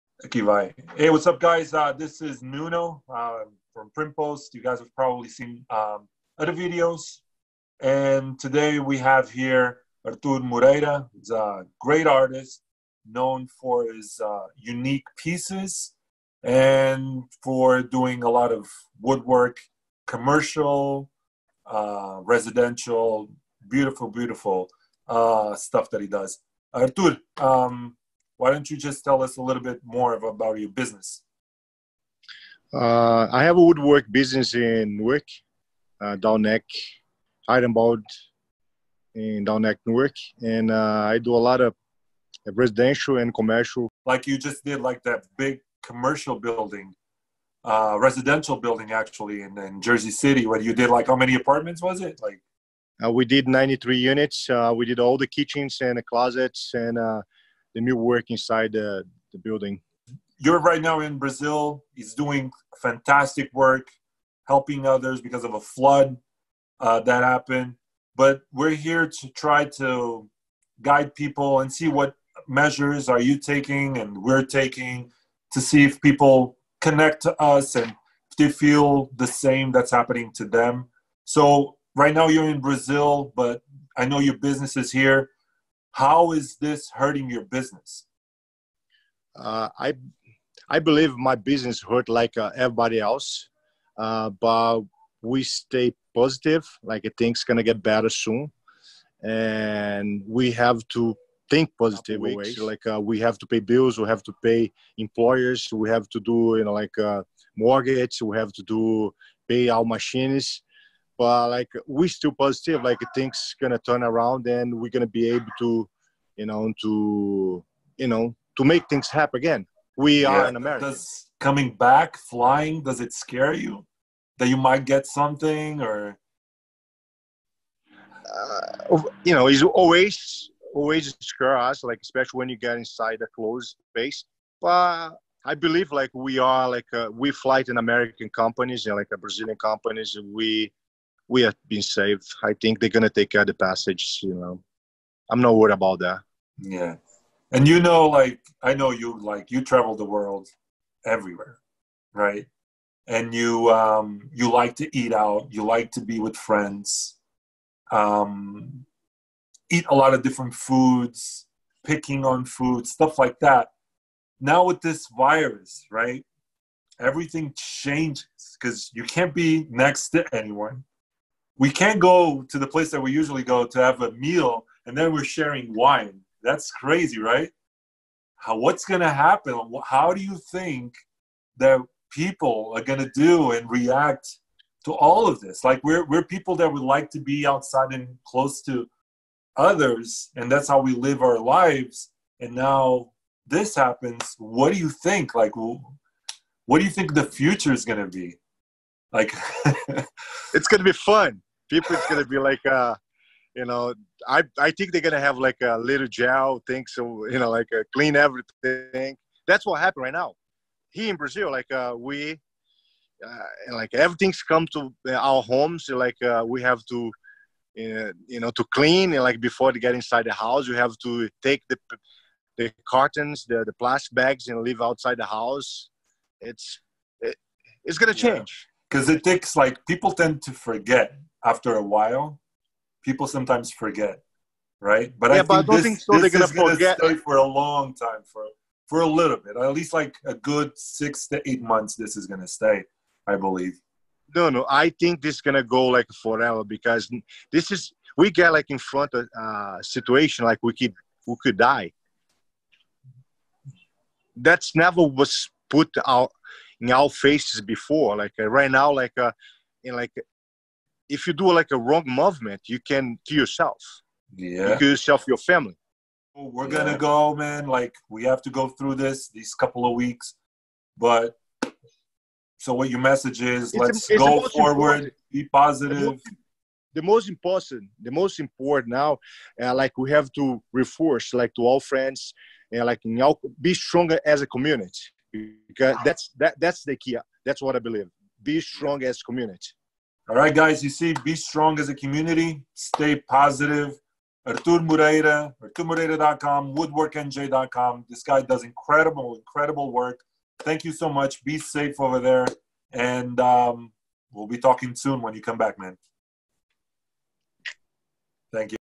Business Vs COVID19 - Construction
Hi on this video we interview @arturmoreira a commercial and residential Woodworker and how his company is dealing with this crisis with some inside tips